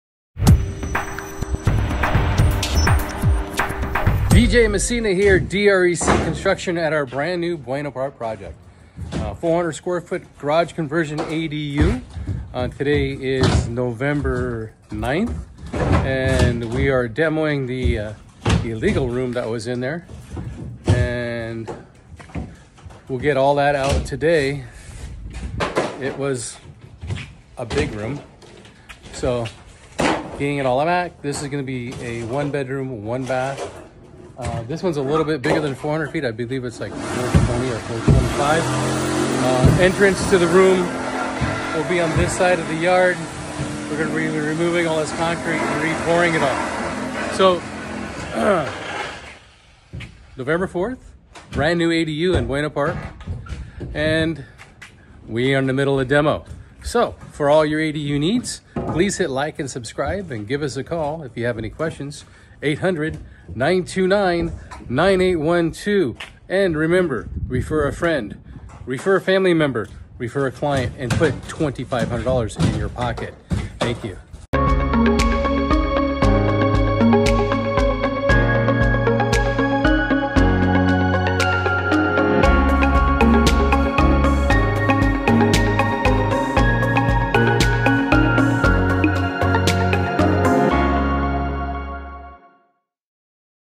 ADU Buena Park | Part 1 - 425 sqft Brand New Build
Welcome to Part 1 of our ADU (Accessory Dwelling Unit) project in Buena Park! In this video, we take you through the process of building a brand new 425 sqft ADU from start to finish. From laying the foundation to installing the finishing touches, you'll see every step of the construction process.

DREC Construction Services, Inc.